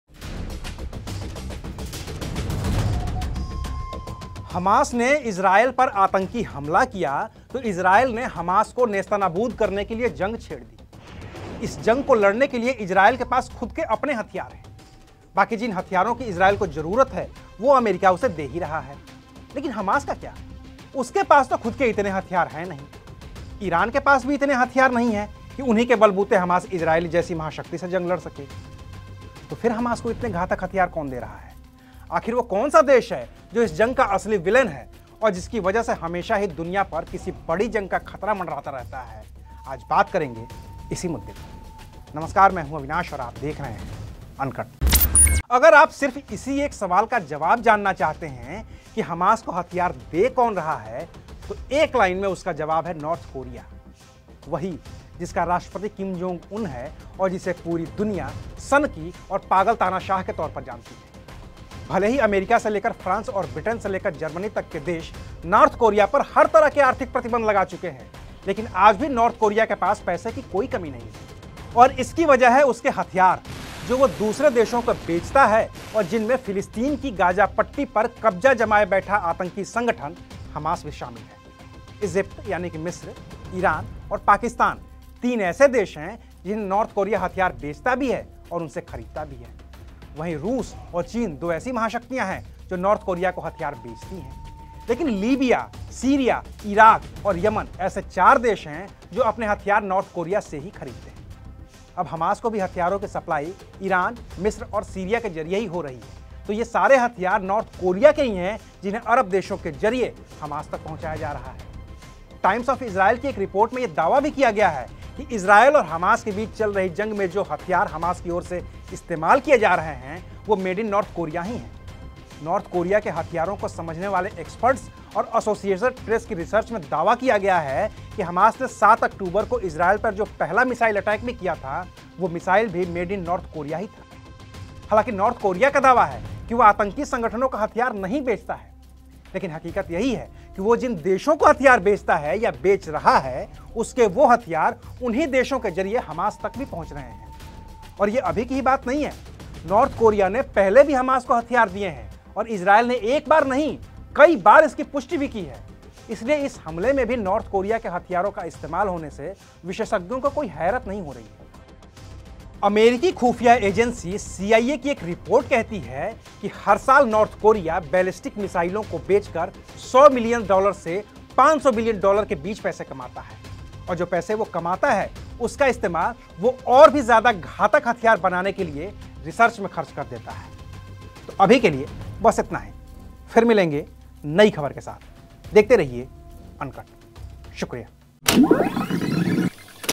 Israel-HAMAS war में North Korea-Kim Jong Un की एंट्री, सीरीया-ईरान से हमास को हथियार दे रहा कोरिया?
हमास ने इज़रायल पर आतंकी हमला किया तो इज़रायल ने हमास को नेस्तनाबूद करने के लिए जंग छेड़ दी. इस जंग को लड़ने के लिए इज़रायल के पास खुद के अपने हथियार हैं. बाकी जिन हथियारों की इज़रायल को जरूरत है, वो अमेरिका उसे दे ही रहा है. लेकिन हमास का क्या. उसके पास तो खुद के हथियार नहीं हैं. ईरान के पास भी इतने हथियार नहीं हैं कि उन्हीं के बलबूते हमास इज़रायल जैसी महाशक्ति से जंग लड़ सके. तो फिर हमास को इतने घातक हथियार दे कौन रहा है. आखिर वो कौन सा देश है, जो इस जंग का असली विलेन है और जिसकी वजह से हमेशा ही दुनिया पर किसी बड़ी जंग का खतरा मंडराता रहता है, बता रहे हैं अविनाश राय.